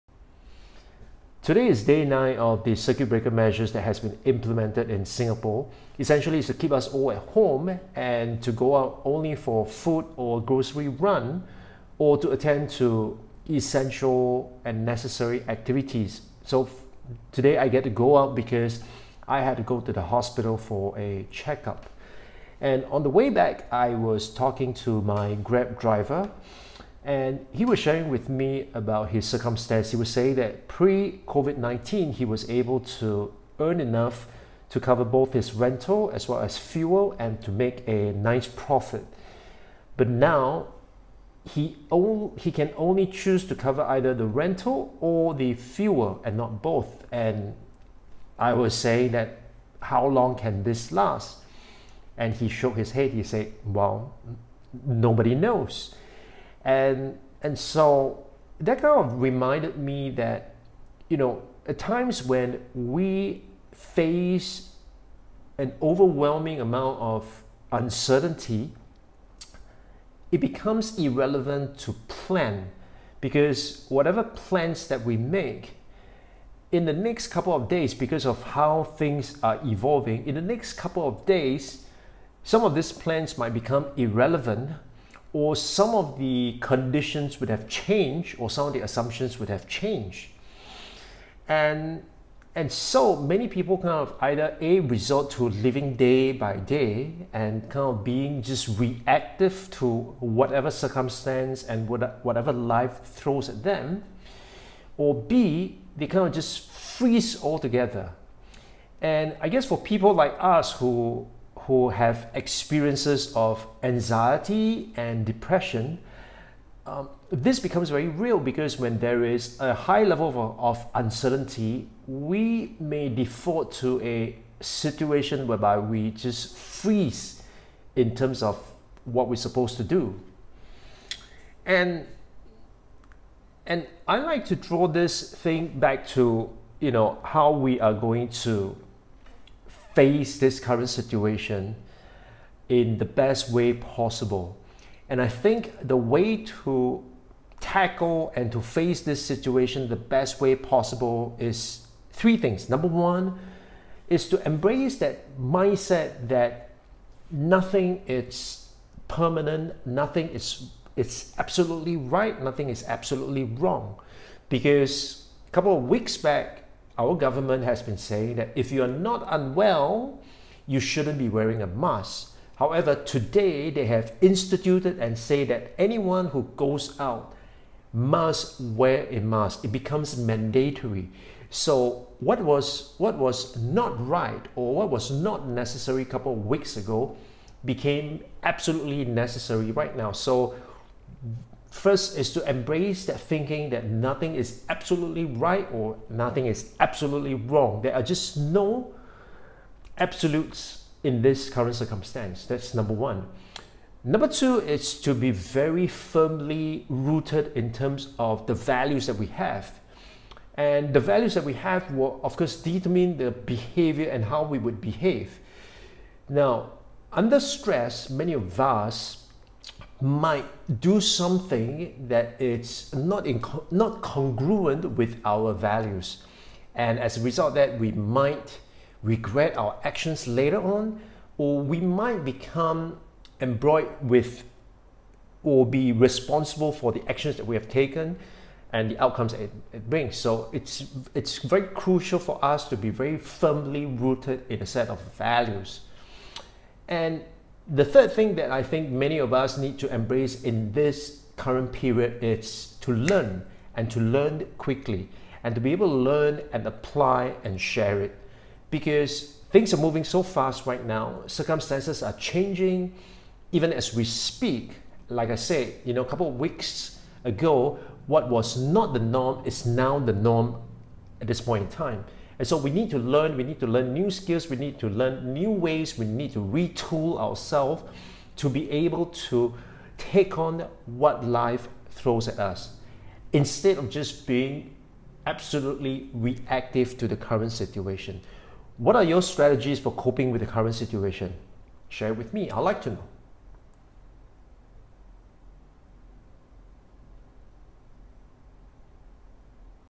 DURING UNCERTAIN TIMES, DON'T PLAN. DO THESE 3 THINGS INSTEAD.
Planning is possible when we are able to hold certain assumptions constant.

However, during uncertain times, such as now, planning becomes difficult or even impossible.

In this video, I share the three things you must do during uncertain times.

🤔 Do you still plan during uncertain times?

🎯 The MelvynLIVE Show is about becoming better every day.